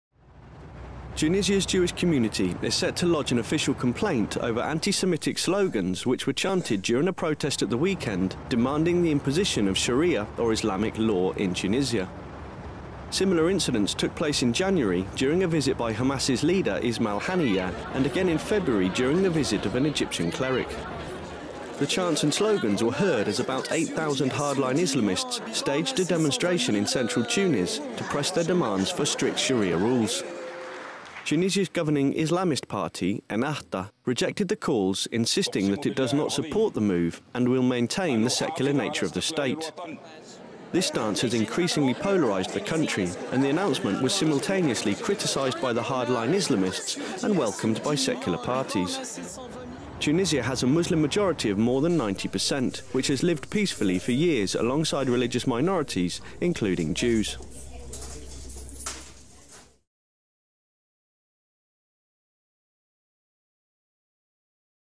8,000 Tunisians Demand Sharia Law while Shouting Anti-Semitic Slogans in Tunis - Ennahda Rejects
Tunisia's Jewish community is filing an official complaint over anti-Semitic slogans which were chanted during a protest at the weekend demanding the imposition of sharia law in Tunisia. Similar incidents occured in January during a visit by Hamas leader Ismail Haniyeh and again in February during the visit of an Egyptian cleric.

Roughly 8,000 Muslims were demonstrating in central Tunis to demand strict Islamic law, but the dominant party in Tunisia's new government, the moderate Ennahda party, rejected these calls and insisted that it will maintain the secular nature of the government. Ennahda's position has increasingly polarised the country, and while secular-leaning citizens praised its response, fundamentalist Muslims criticised it. Tunisia has an Islamic majority of 90% with relatively peaceful relations with its Jewish minority.